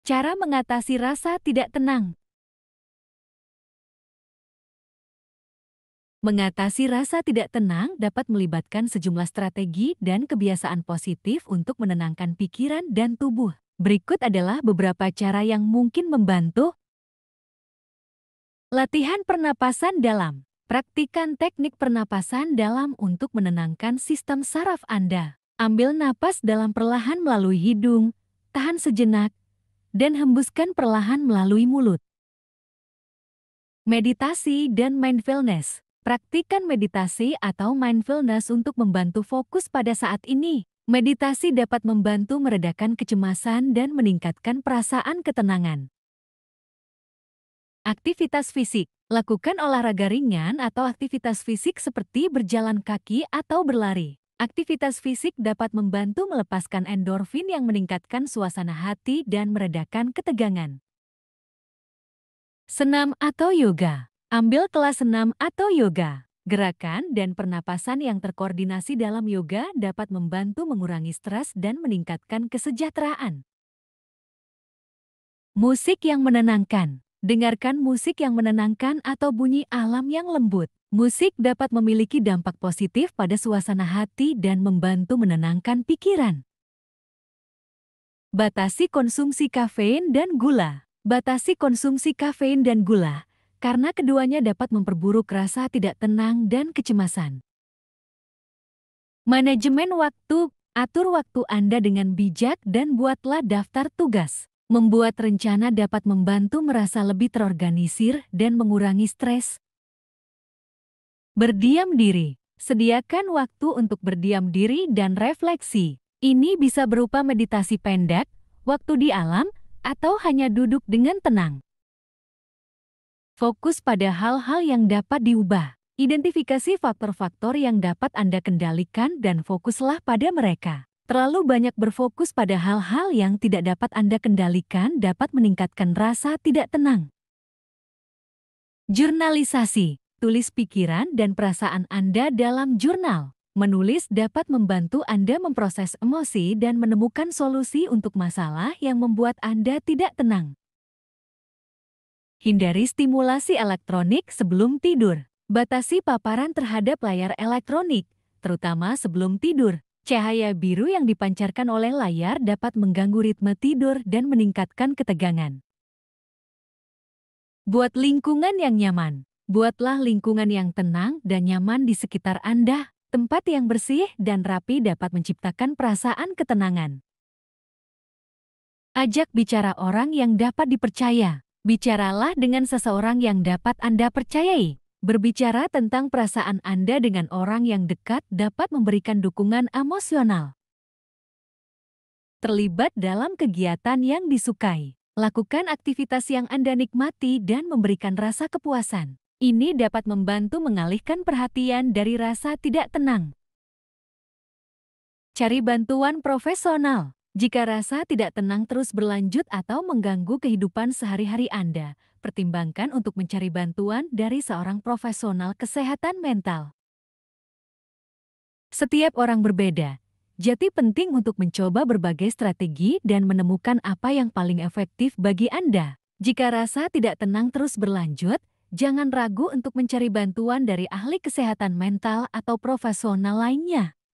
cara mengatasi rasa tidak tenang
Mengatasi rasa tidak tenang dapat melibatkan sejumlah strategi dan kebiasaan positif untuk menenangkan pikiran dan tubuh. Berikut adalah beberapa cara yang mungkin membantu: